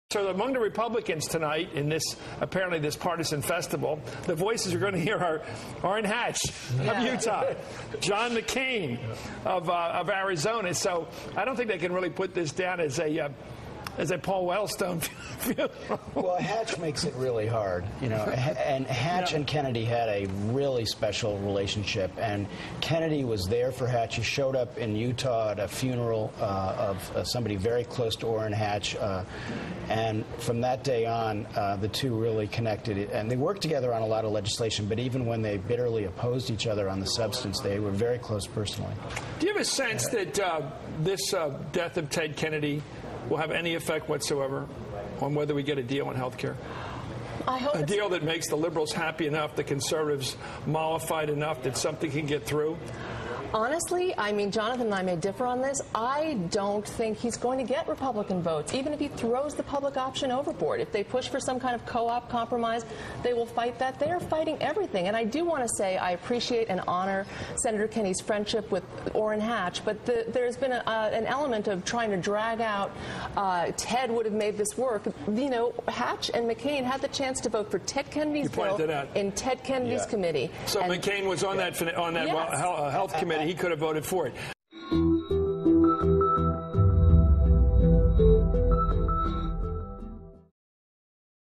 Joan Walsh: Hatch & McCain Had the Chance to Vote for Kennedy's Health Bill
) Walsh: Hatch and McCain "had the chance to vote for Ted Kennedy's bill in Ted Kennedy's committee"